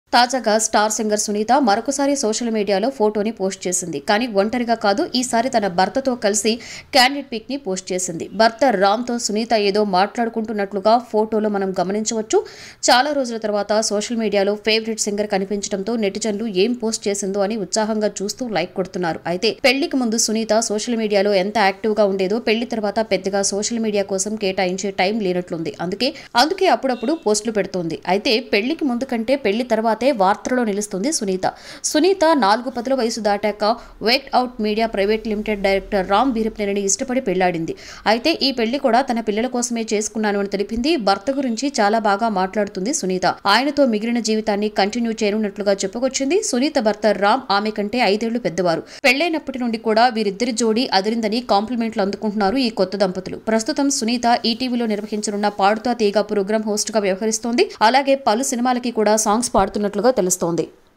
భర్త రామ్ తో సునీత లేటెస్ట్ పిక్ | Singer Sunitha Shares Her Latest Photo With Husband | Santosham
భర్త రామ్ తో సునీత లేటెస్ట్ పిక్ | SingerSunitha Shares Her Latest Photo With Husband | Santosham Film News